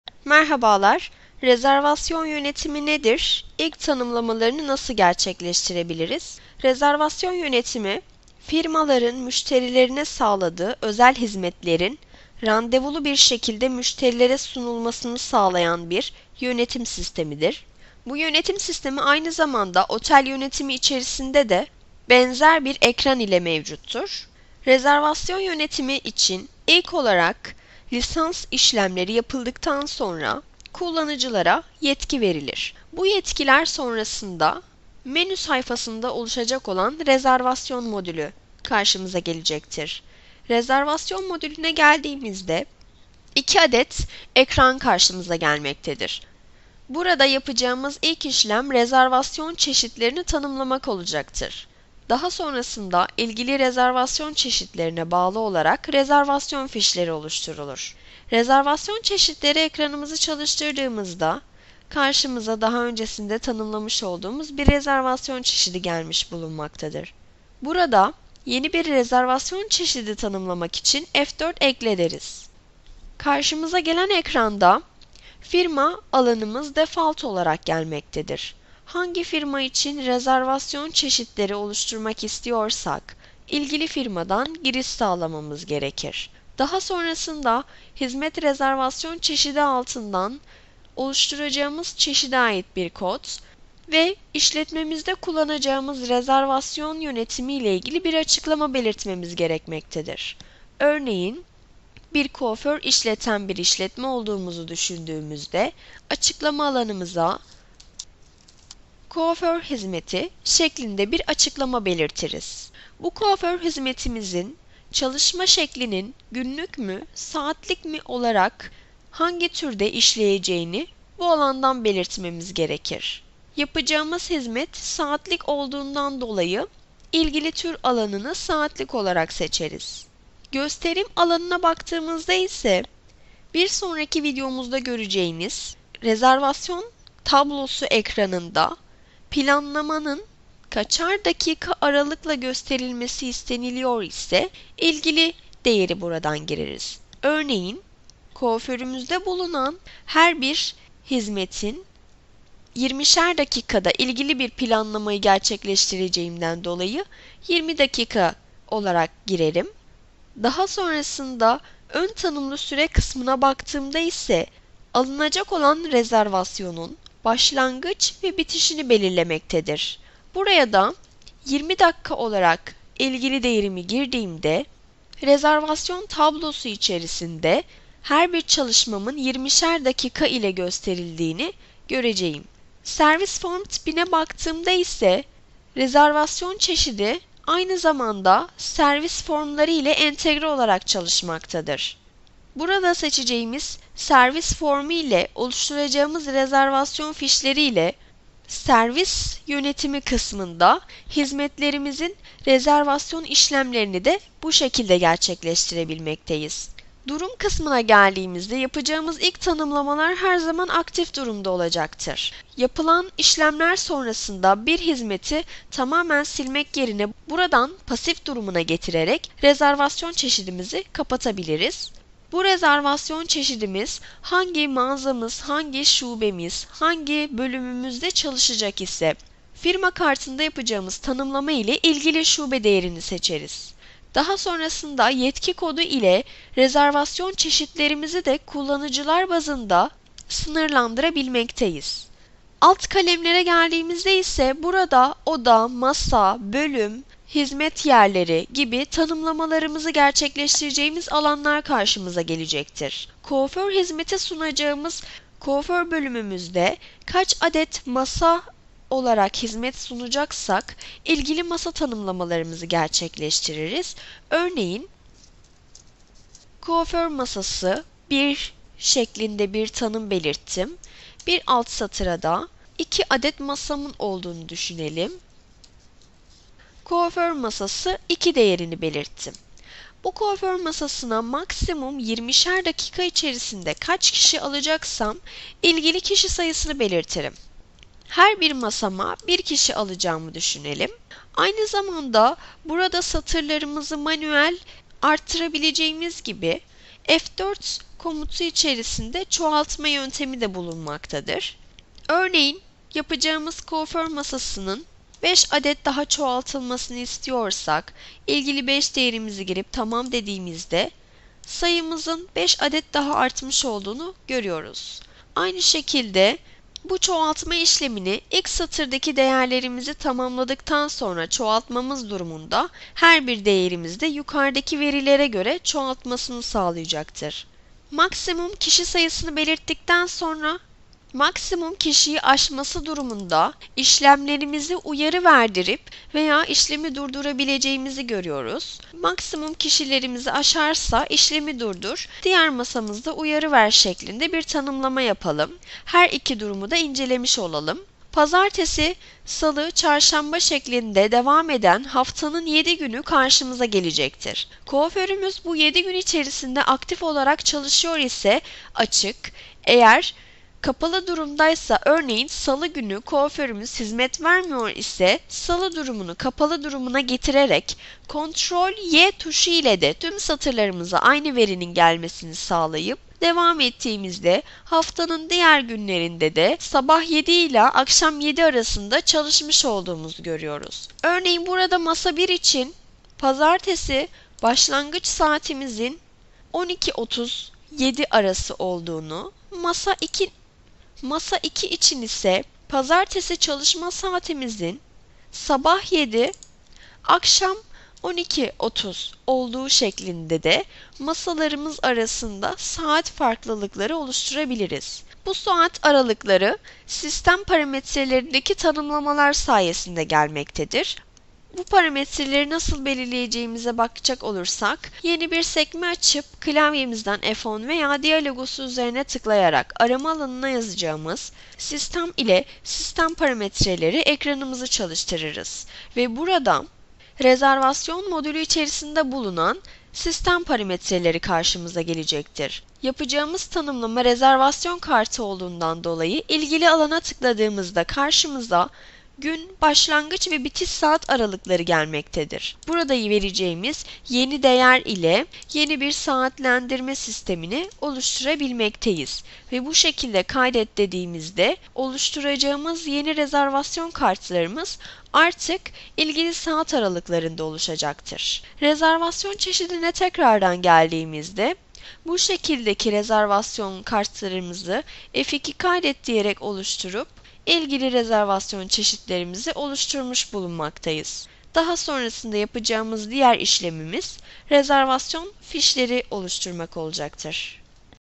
DİA Akademi - Hizmet Rezervasyon Yönetimi Nedir? İlk Tanımlamalar Nasıl Yapılır?
DİA Akademi - Bu videoda hizmet rezervasyon yönetiminin ne olduğu ve ilk tanımlamaların nasıl yapıldığı anlatılmaktadır.